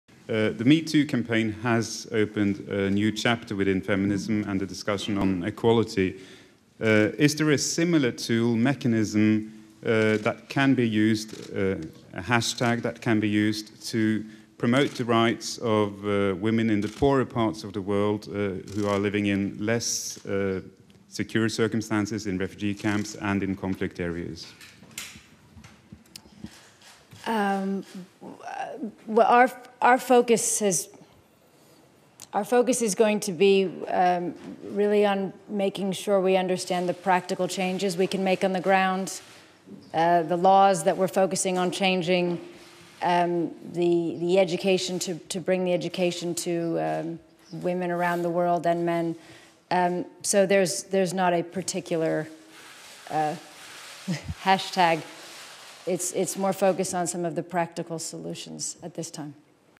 Angelia Jolie: "Hashtag come MeToo per combattere crimini di guerra? Servono misure concrete"
(Agenzia Vista) Roma, 01 febbraio 2018
Angelia Jolie Un hashtag come MeToo per combattere crimini su donne Servono misure concrete
“Un hashtag come MeToo per combattere le violenze di guerra sulle donne? Il nostro obiettivo sono misure concrete per portare un reale cambiamento”. Cosi' l'attrice e regista americana Angelina Jolie, nella veste di ambasciatrice di buona volontà dell'Onu, ha risposta alla domanda di un giornalista nel corso della sua visita al quartier generale della Nato a Bruxelles. Jolie ha annunciato che collaborerà, "nei mesi e anni a venire", con la Nato per rafforzare la lotta alle violenze sessuali nei conflitti. E a chi gli chiedeva se in questo progetto userà una campagna di sensibilizzazione come quella di MeToo, ha risposto seccamente ma col sorriso: “Il nostro obiettivo è comprendere quali cambiamenti pratici possiamo apportare sul campo, le leggi su cui concentrarsi per portare questo cambiamento e l'istruzione alle donne e agli uomini nel mondo. Quindi non c'è un hashtag particolare. E' più focalizzato su soluzioni pratiche in questo momento”.